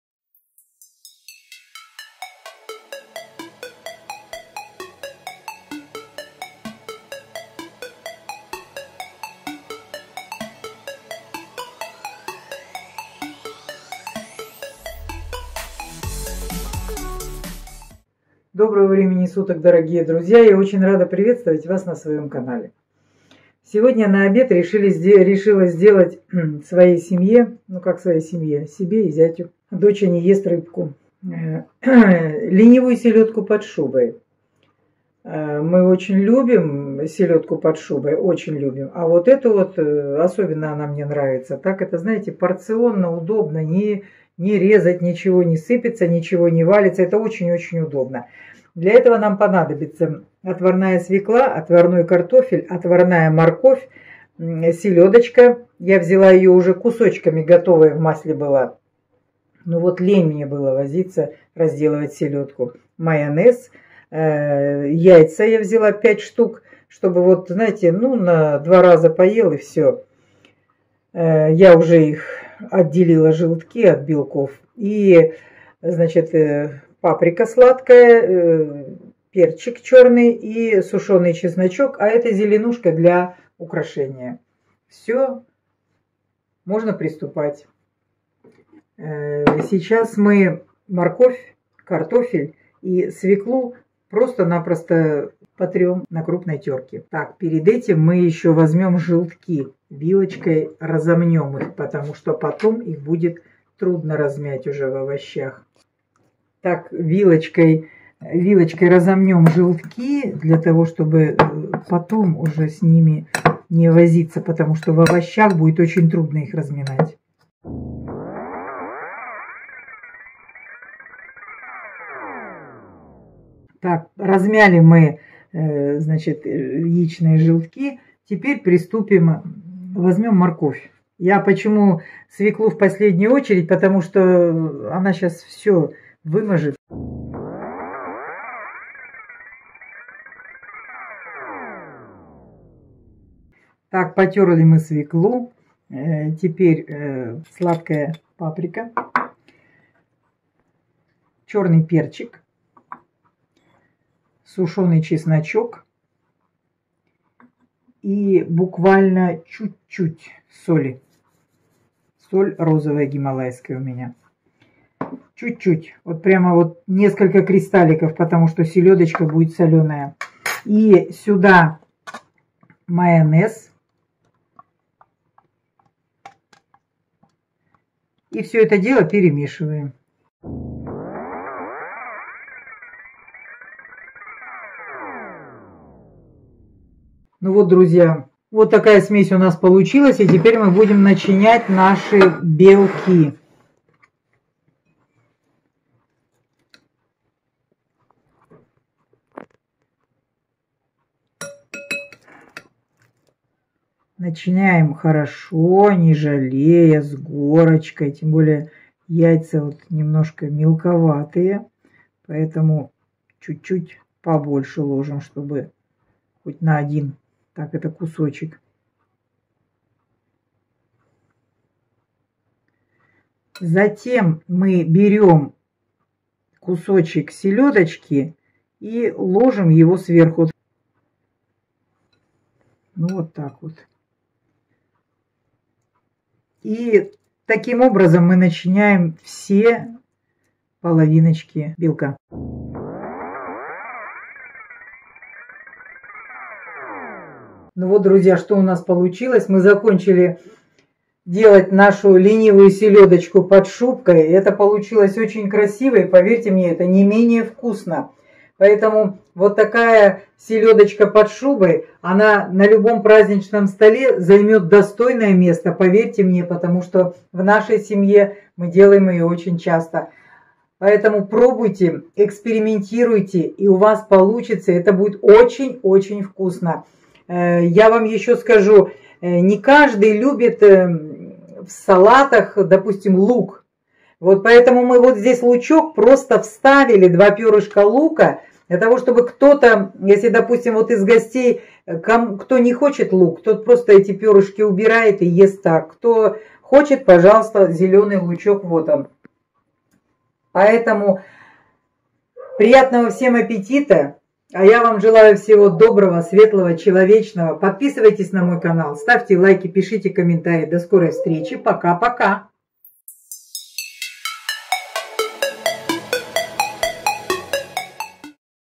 ЛЕНИВАЯ "СЕЛЁДКА ПОД ШУБОЙ"
Сегодня  я хочу вам показать наш любимый салат Ленивая "Селедка под шубой". Это очень простой и вкусный салат, на приготовление которого у вас уйдёт минимум времени, при условии, что овощи вы отварите заранее. Попробуйте вашу любимую селёдочку в другой интерпретации и вы будете приятно удивлены.